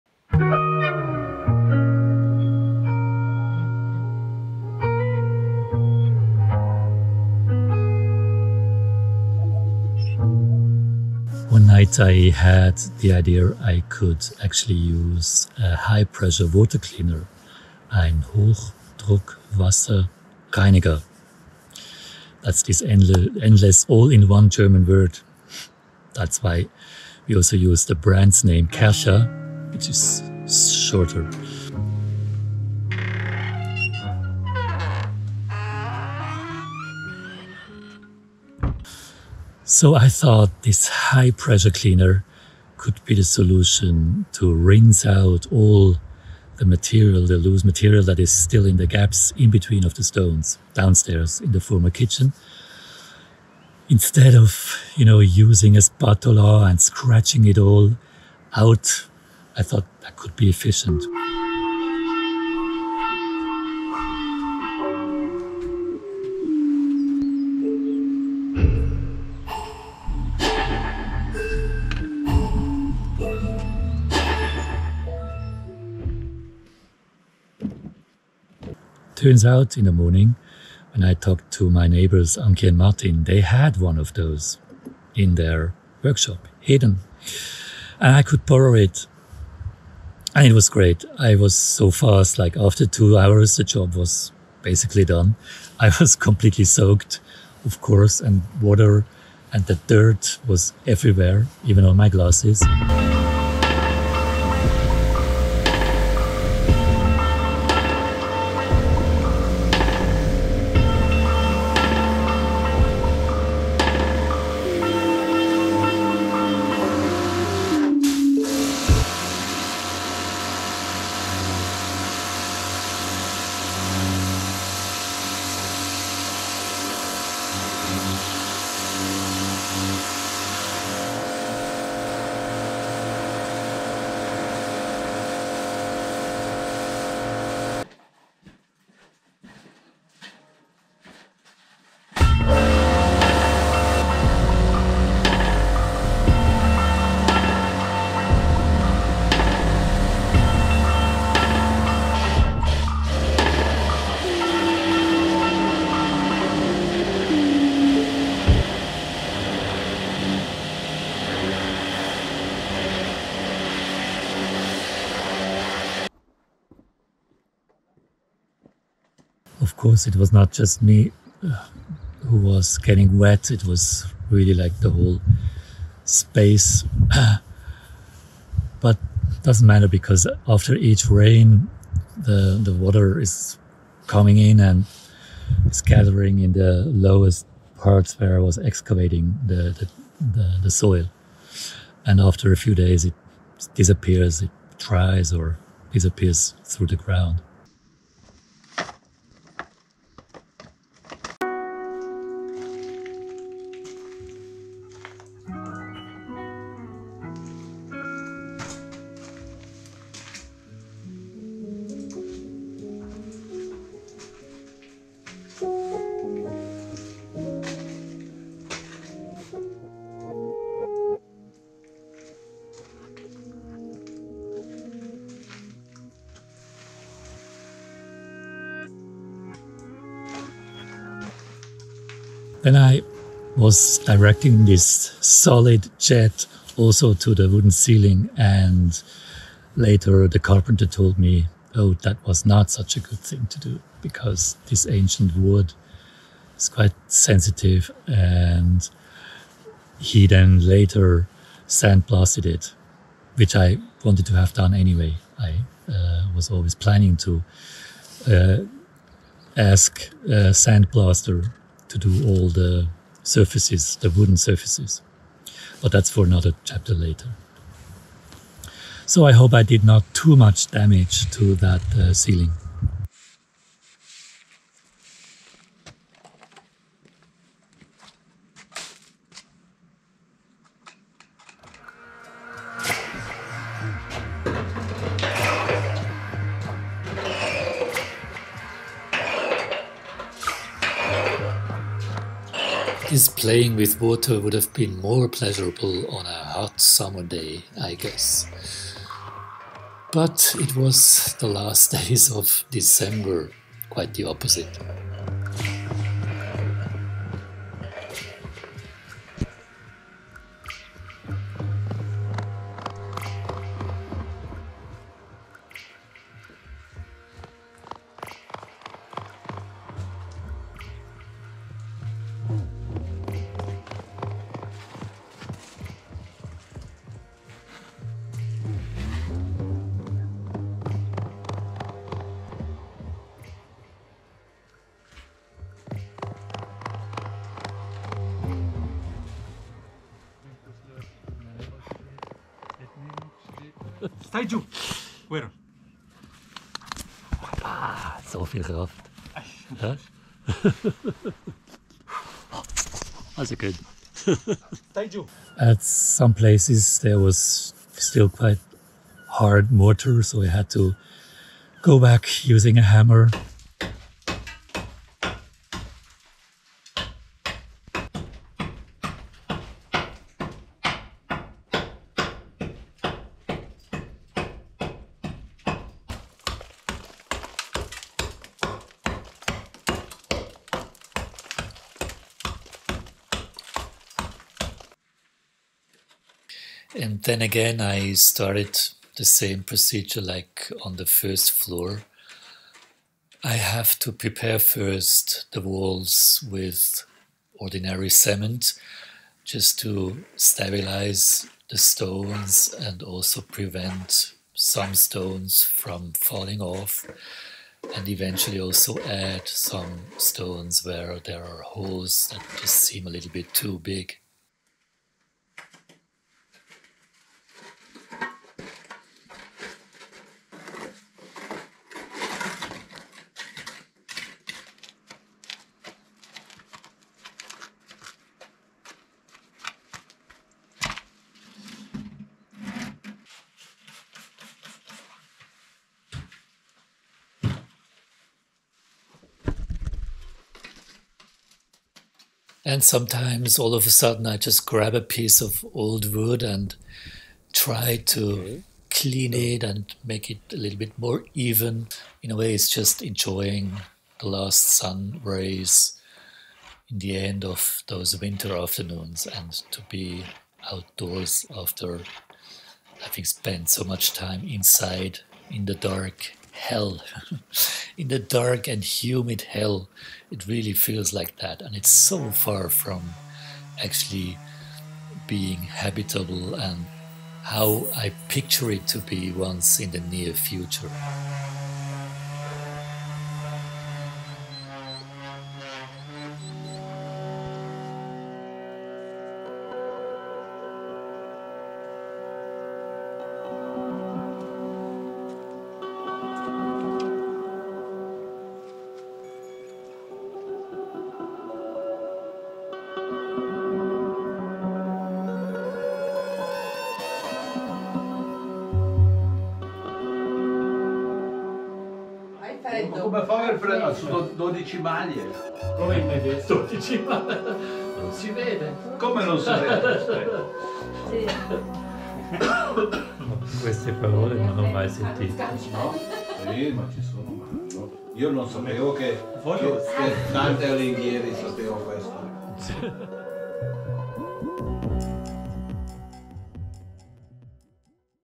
#07 Cleaning stone walls with a high pressure water cleaner. I Christoph Felix
Renovating a 140 year old Stone House in the Italian Alps.
Instead of time-consuming fine work with a spatula, a high-pressure device can be useful. The joints between the stones are efficiently cleaned of smaller particles and mortar residues. Accordingly, the chaos is then everywhere else: the water spreads all the dirt on the floor, on windows, on the ceiling, and so on. This stage is really the furthest from any habitability...kind of the nadir of the reconstruction!